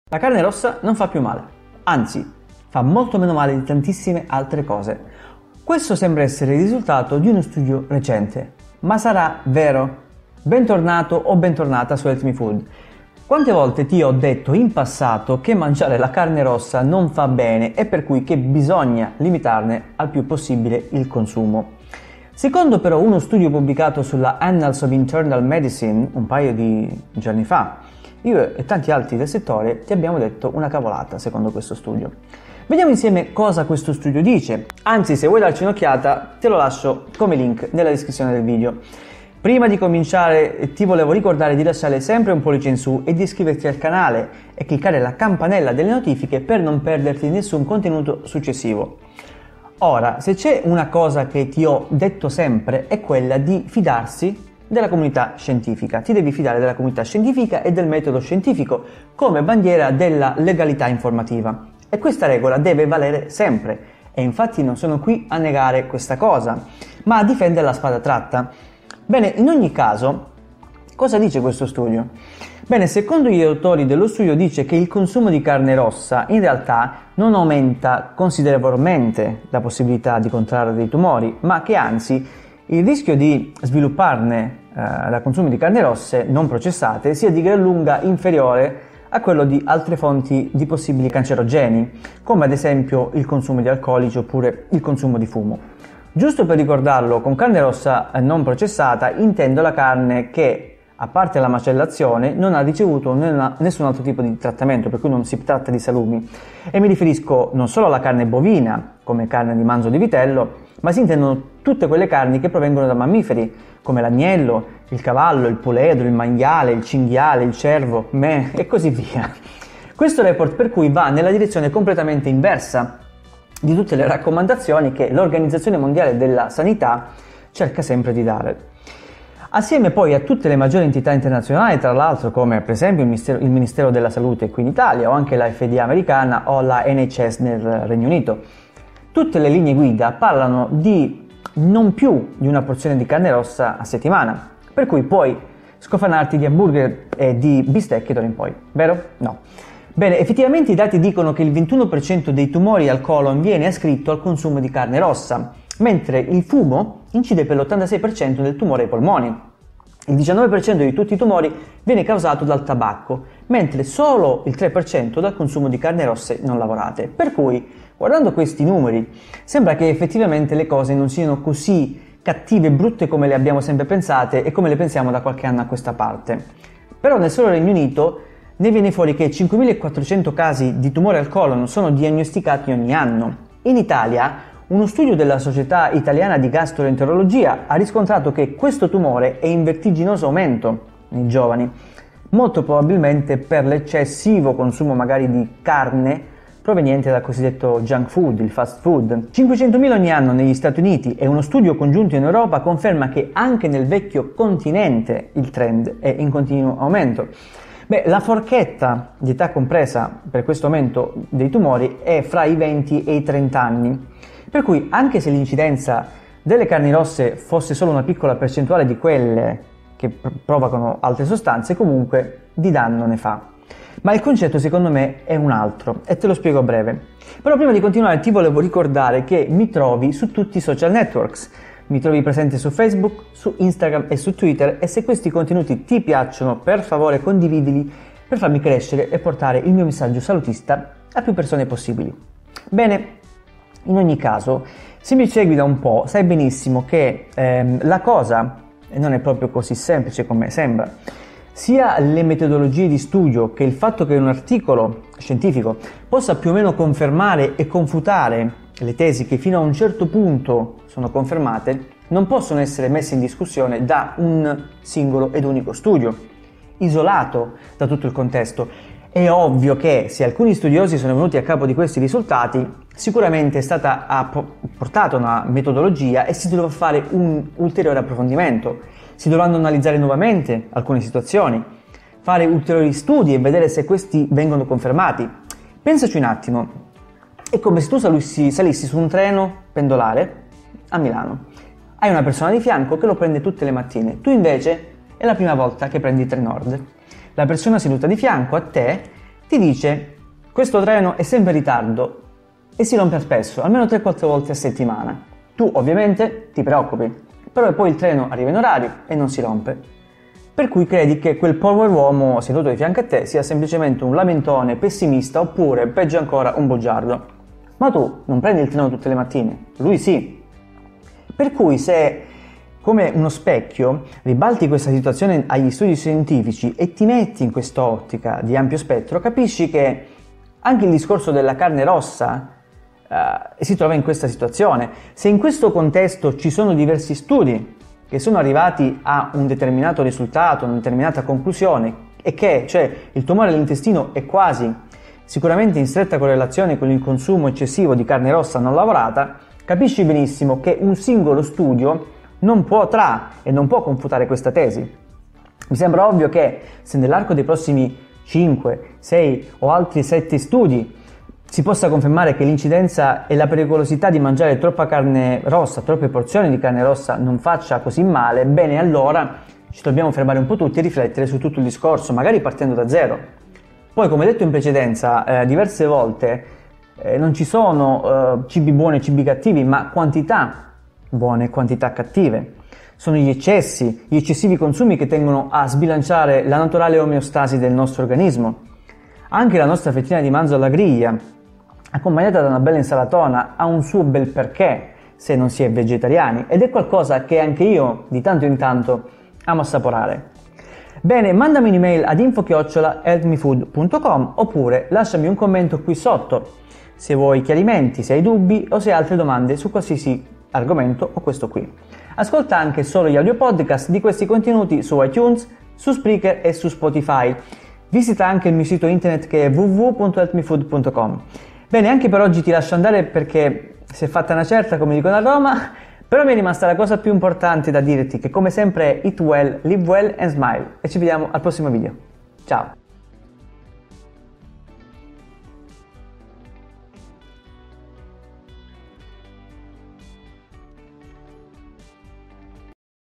Episodio 71 - LA CARNE ROSSA NON FA MALE? - Discutiamo un nuovo studio a tal proposito
Social e Website

PROJECT NUTRITION
GUIDA ALIMENTAZIONE SPORTIVA
BUFALE SUL CIBO
MITI ALIMENTARI

La carne rossa non fa più’ male. Anzi fa molto meno male di tantissime altre cosa. Questo sembra essere il risultato di uno studio recente. Ma sara’ vero?

Quante volte di ho detto in passato, che mangiare la carne rossa non fa bene e per cui che bisogna limitarne al più’ possibile il consumo. Secondo pero’ uno studio pubblicato sul Annals of Internal Medicine un paio di giorno fa, io e tanti altri del settore, ti abbiamo detto una cavolata. Vediamo insieme cosa questo studio dice.

Ora se c’e’ una cosa che ti ho detto sempre, e’ quella di fidarsi della comunità’ scientifica e del metodo scientifico, come bandiera della legalità’ informativa. E questa regola deve valere sempre, ed infatti non sono qui a negare questa cosa, ma a difenderla a spada tratta. In ogni caso cosa dice lo studio? Secondo gli autori, lo studio dice che il consumo di carne rossa non aumenta considerevolmente le possibilità’ di contrarre tumori. Ma che anzi i rischio di svilupparne da consumo di carni rosse non processate sia di gran lunga inferiore a quello di altre fonti di possibili cancerogeni, come ad esempio il consumo di alcolici oppure il fumo.

Giusto per ricordarlo, con carne rossa non processato intendo la carne che a parte la macellazione, non ha ricevuto nessun altro tipo di trattamento. E mi riferisco non solo alla carne bovina, come carne di manzo o di vitello. Ma si intendono sempre tutti i mammiferi come agnello, cavallo, puledro, maiale, cervo, cinghiale e cosi via. Questo report per cui, va nella direzione completamente inversa, di tutte le raccomandazioni, che l’organizzazione mondiale della sanità’, cerca di dare. Assieme poi a tutte le maggiori entità’ interazionali, come il nostro ministero dalla salute, o la FDA americana o la NHS nel regno unito. Tutte le linee guida parlano di non piu’ di una porzione di carne rossa a settimana. Per cui puoi scofanarti di hamburger da ora in poi?

Effettivamente i dati dicono che il 21% dei tumori al colon viene ascritto al consumo di carne rossa, mentre i il fumo incide per l’86% dei tumori ai polmoni. Il 19% di tutti i tumori viene causato dal tabacco, mentre solo il 3% dal consumo di carni rosse non lavorate. Per cui guardando questi numeri, sembra che effettivamente le cose non siano cosi come le pensiamo da un po di anni a questa parte. Nel solo regno unito pero’, ne viene fuori che 5400 casi di tumore al colon, sono diagnosticati ogni anno. In Italia, uno studio della società’ italiana di gastroenterologia, ha riscontrato che questo tumore e’ in vertiginoso aumento nei giovani, molto probabilmente per l’eccessivo consumo di carne proveniente dal cosiddetto junk food. Il fast food. 500 mila ogni anno negli stati uniti, ed uno studio congiunto in europa, conferma che anche nel vecchio continente il trend e’ un aumento.

La forchetta’ di eta’ e’ compresa fra i 20 ed i 30 anni. Per cui anche se l’incidenza della carne rossa fosse solo una piccola percentuale di quella di altre sostanze, comunque di danno ne fa.